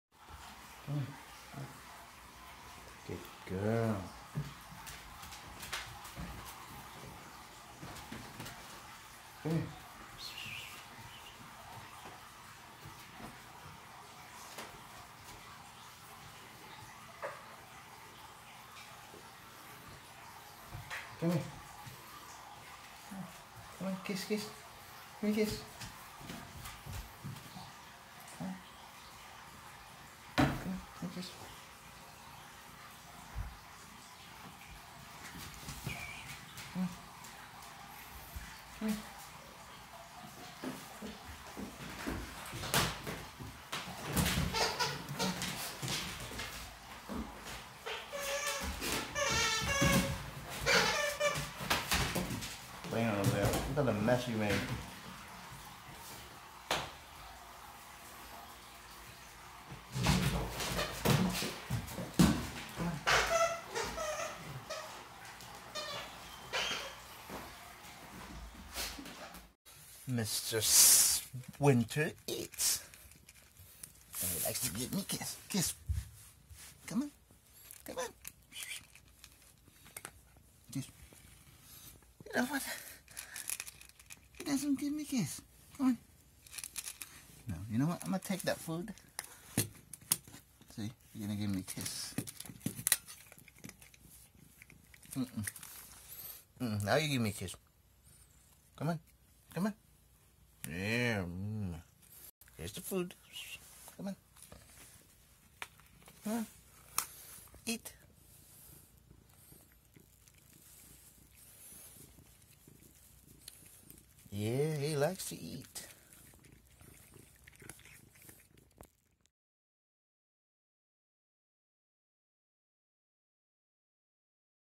Affectionate Cat - Mr. WhiteFace aka Mr. Winter Snow
My very affectionate Cat Mr. White Face.

Ever since we got him as a kitten he was very affectionate towards us but not to strangers. haha